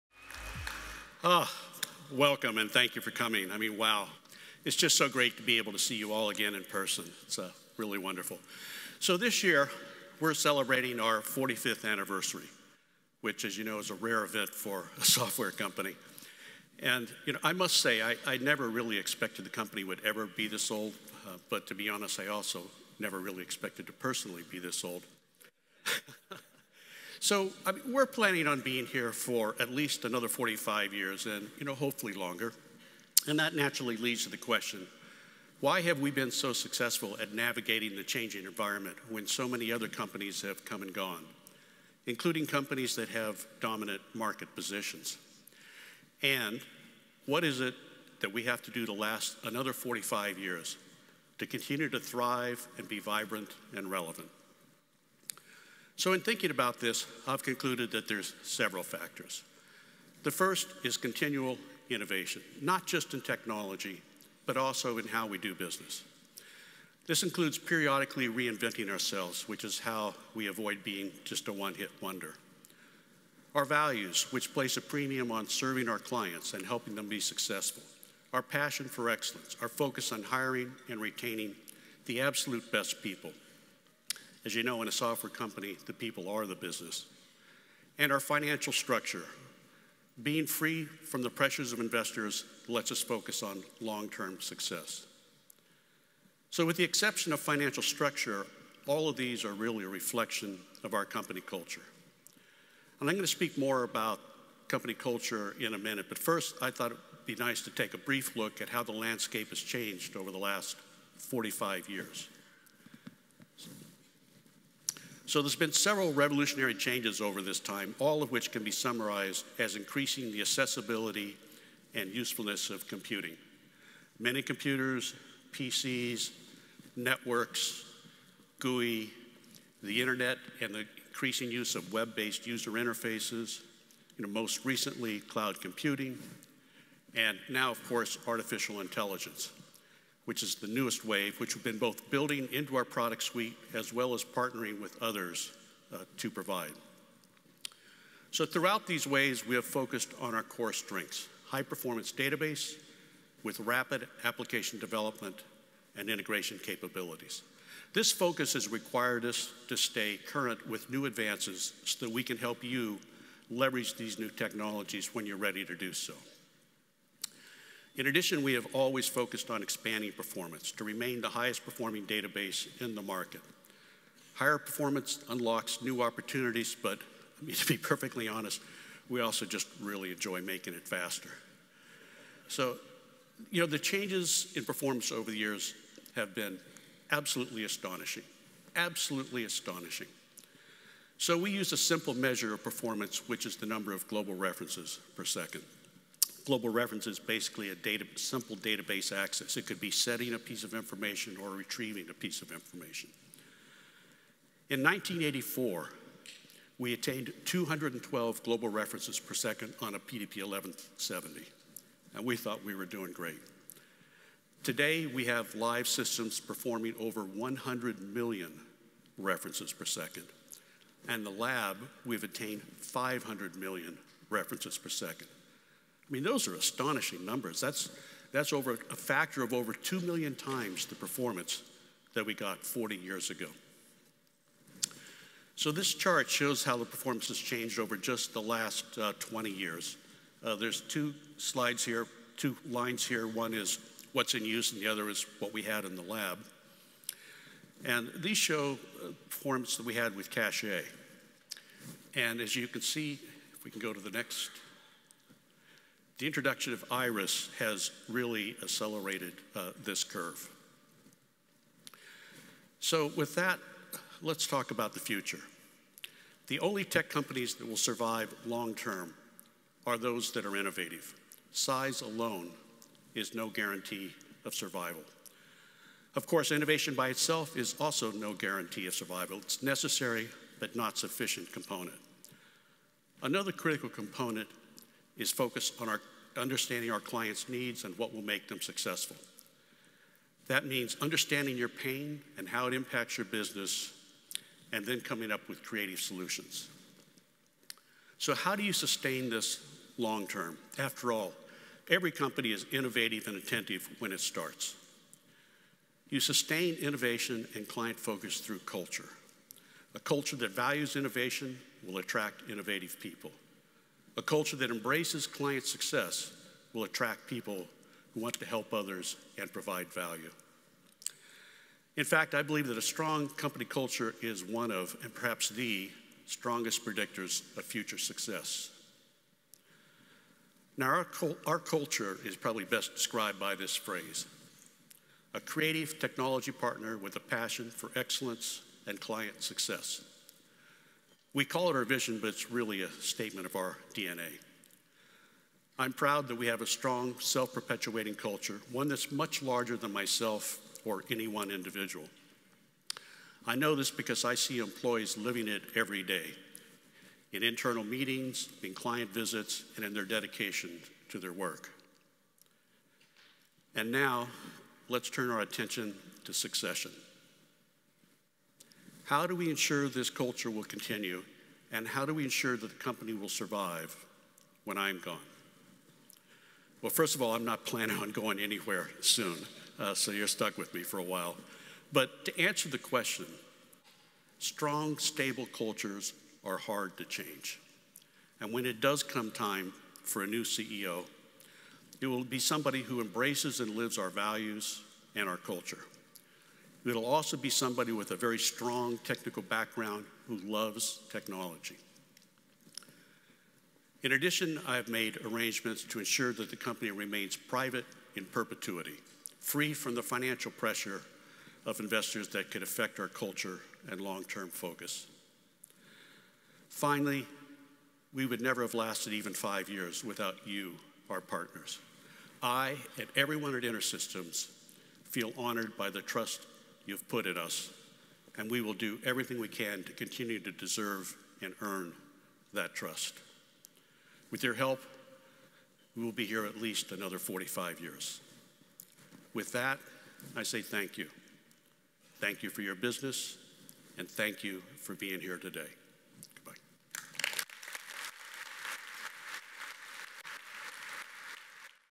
Continuing Excellence
Keynote from Global Summit 2023 presented by Terry Ragon